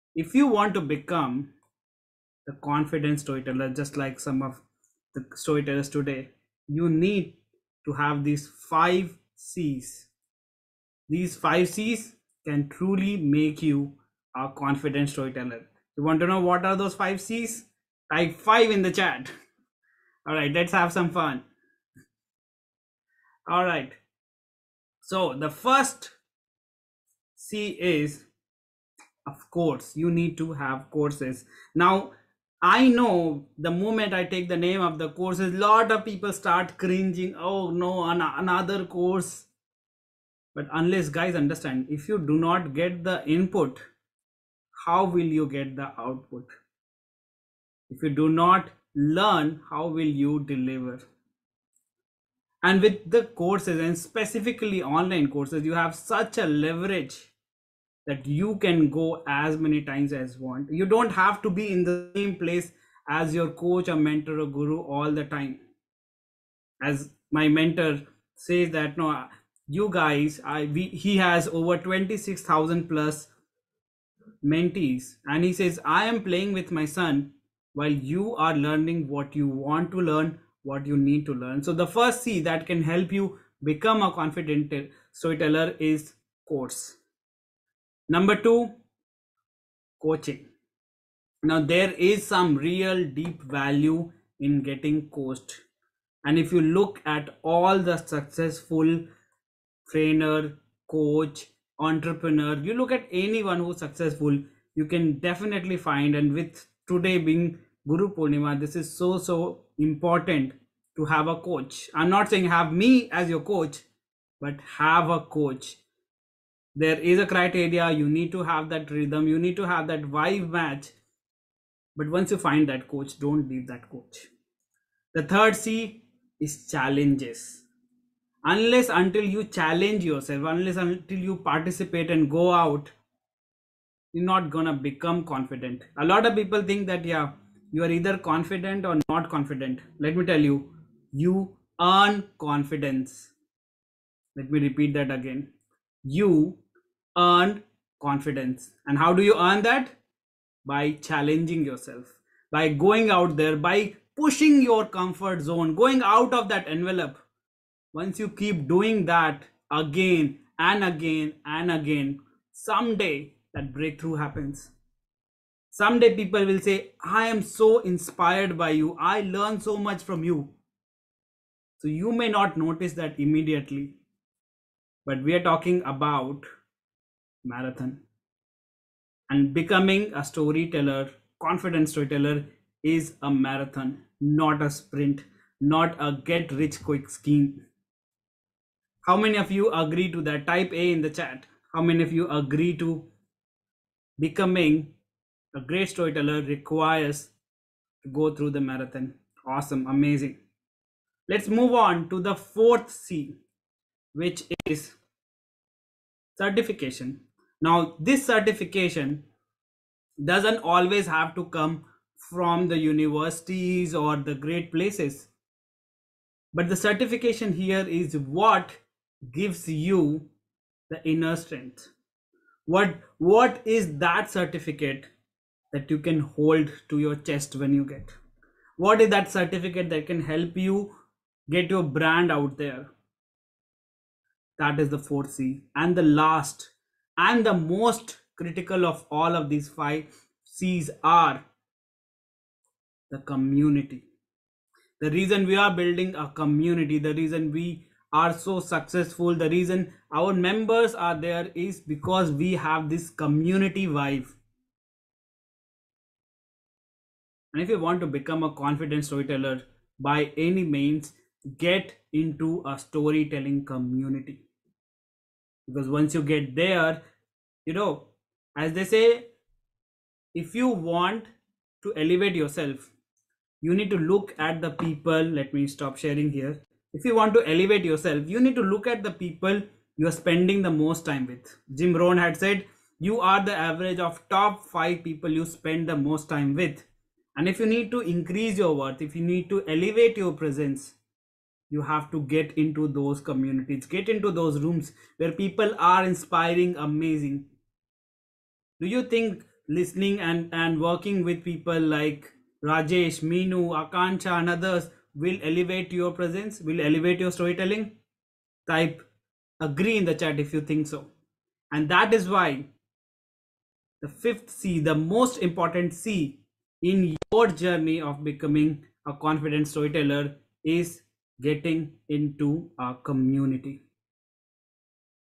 5 Cs to beocme Confident Storyteller
In this video, I'm going to teach you 5 Cs to be a confident Storyteller. These 5 Cs will help you develop your storytelling skills and increase your confidence as a Storyteller.

As a storyteller, it's essential to have a strong foundation in your craft. In this video, I'm going to teach you the 5 Cs that will help you build a strong foundation in storytelling. These 5 Cs are Character, Composition, Control, Coherence, and Clarity. By following these principles, you'll be on your way to becoming a confident Storyteller!